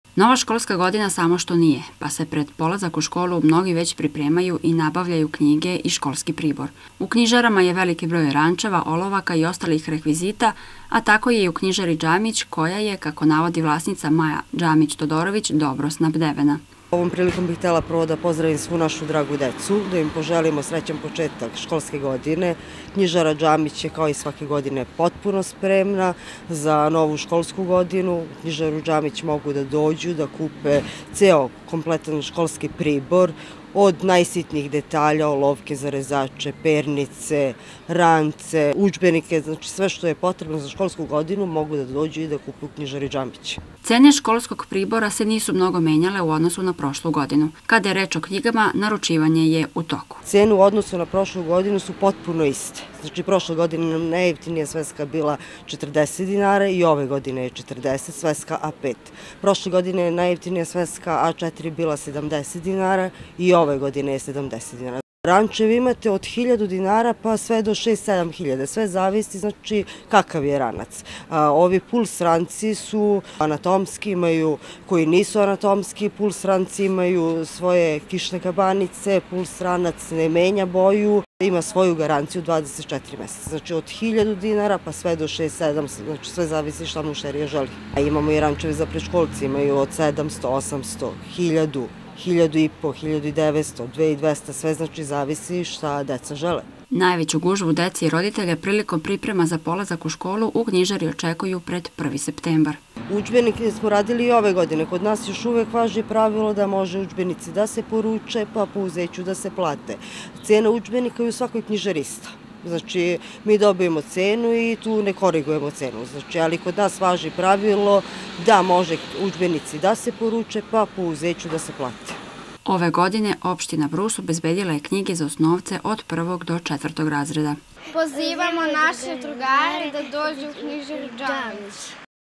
U knjižari Džamić spremni za početak školske godine
Nova školska godina samo što nije, pa se, pred polazak u školu mnogi već pripremaju, nabavljaju knjige i školski pribor. Knjižara Džamić u Brusu snabdevena je kompletnim školskim priborom a cene su gotovo nepromenjene u odnosu na prošlu godinu, navodi vlasnica Maja Džamić Todorović. Najjeftinija mala sveska košta 40 a sveska A4 formata od 70 do 240 dinara.  Za ranac treba izdvojiti od 1.000 do 7.000 dinara, a njihova garancija je 24 meseca.
U Knjižari Džamić moguća je i porudžbina udžbenika a plaćanje je pouzećem.
Ove godine, opština Brus obezbedila je knjige za osnovce od prvog do četvrtog razreda.